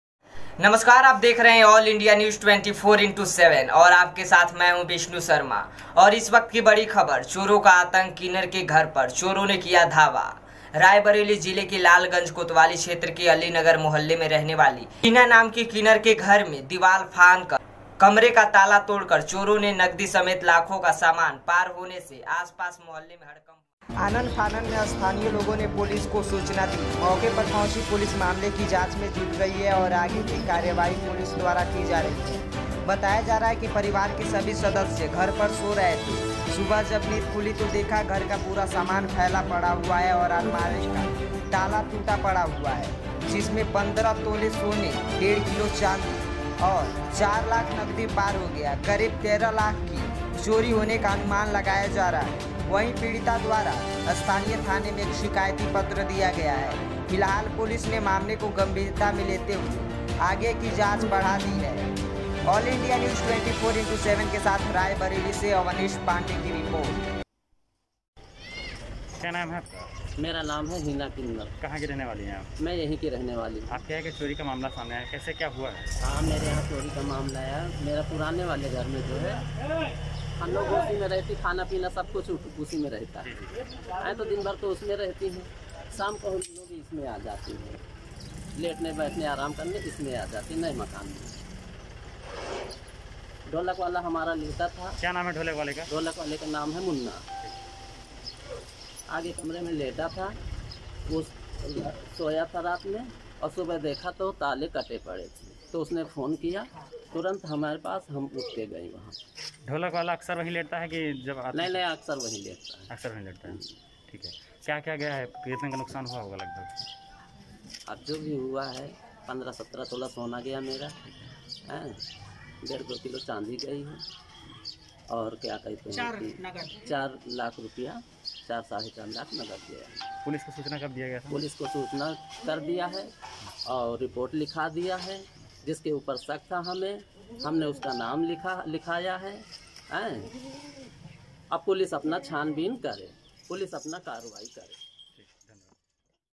चोरों का आतंक, किन्नर के घर पर चोरो का धावा
लोकेशन- लालगंज,रायबरेली

एंकर-रायबरेली जिले के लालगंज कोतवाली क्षेत्र के अलीनगर मोहल्ले की रहने वाली हिना नाम की किन्नर के घर में दीवाल फांद कर कमरे का ताला तोड़कर चोरों ने नगदी जेवर समेत लाखों का सामान पार होने से आसपास मोहल्ले में हड़कंप मच गया आनन-फानन में स्थानीय लोगों ने पुलिस को सूचना दी मौके पर पहुंची पुलिस मामले की जांच में जुट गई है और आगे की कार्यवाही पुलिस द्वारा की जा रही है बताया जा रहा है। परिवार के सभी सदस्य घर पर सो रहे थे सुबह जब नींद खुली तो देखा घर का पूरा सामान फैला पड़ा हुआ है और अलमारी का ताला टूटा पड़ा हुआ है जिसमें 15 तोला सोना डेढ़ किलो चांदी और ₹40,0000 नकदी पार हो गया है करीब 13 लाख की चोरी होने का अनुमान लगाया जा रहा है ।वही पीड़ित द्वारा स्थानीय थाने में एक शिकायती पत्र दिया गया है फ़िलहाल पुलिस ने मामले को गंभीरता से लेते हुए आगे की जांच बढ़ा दी गई है।
बाइट- हीना किन्नर लालगंज रायबरेली